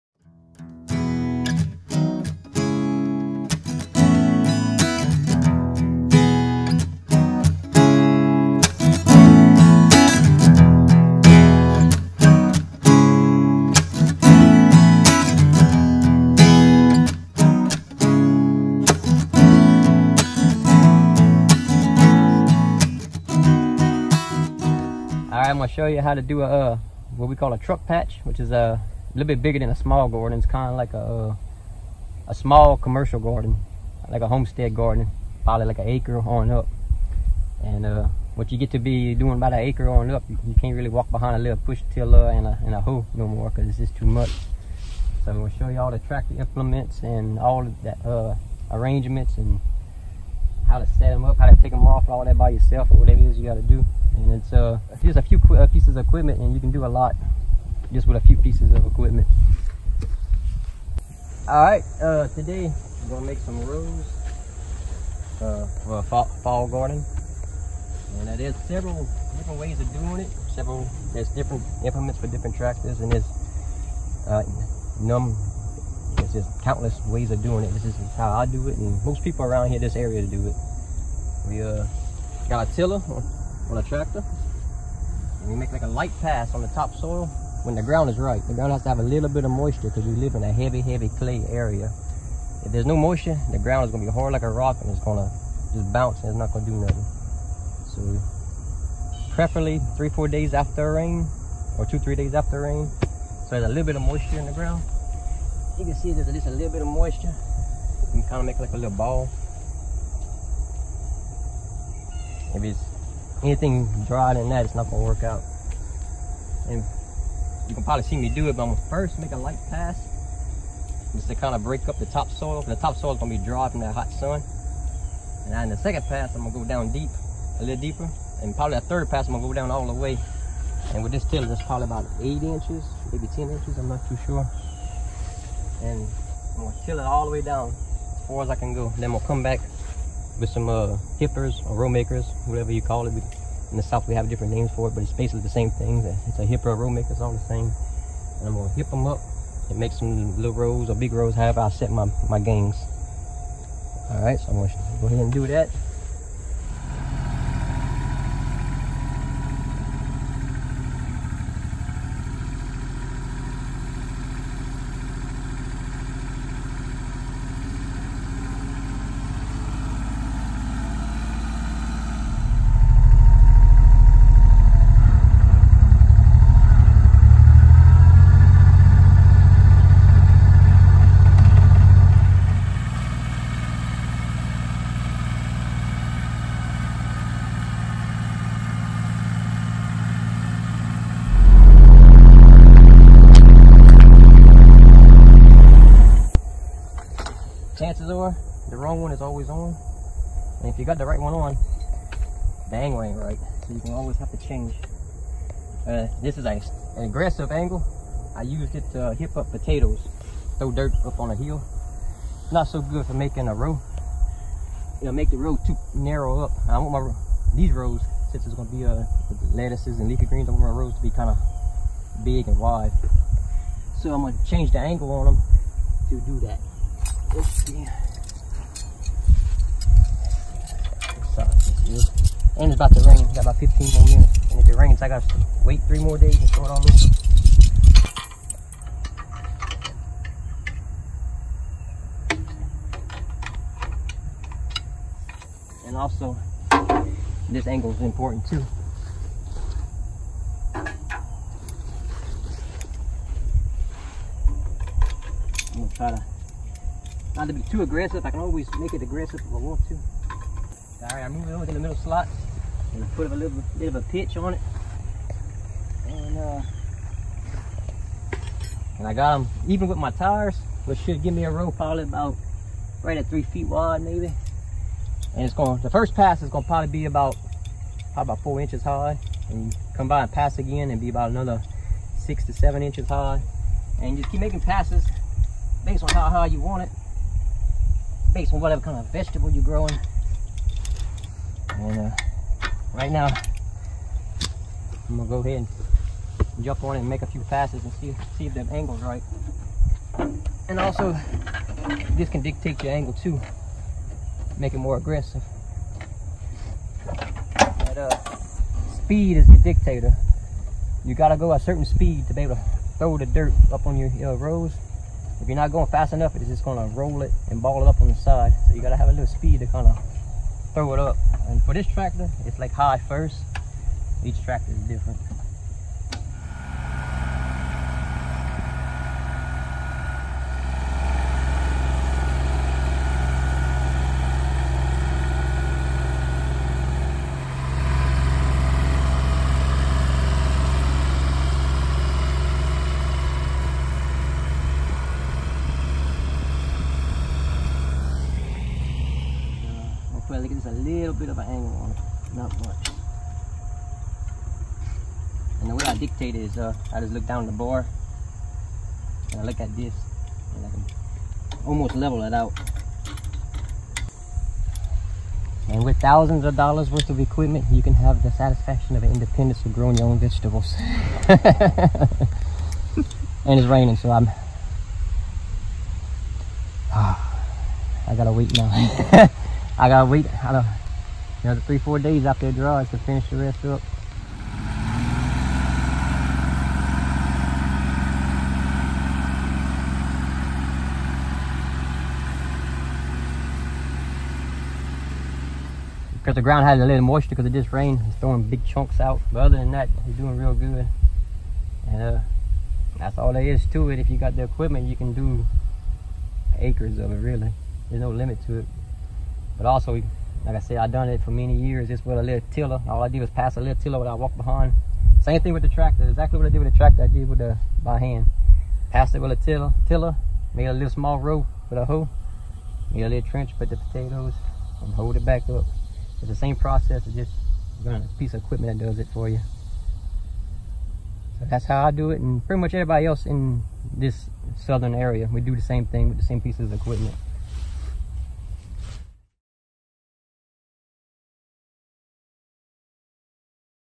Market Gardening : Preparing Your Ground
This is the first video in a series on market (or homestead) gardening. In this video we begin the process by preparing the ground and making our rows!

***Sorry I had to re-upload this video. The audio didn’t load right the first time I uploaded it :(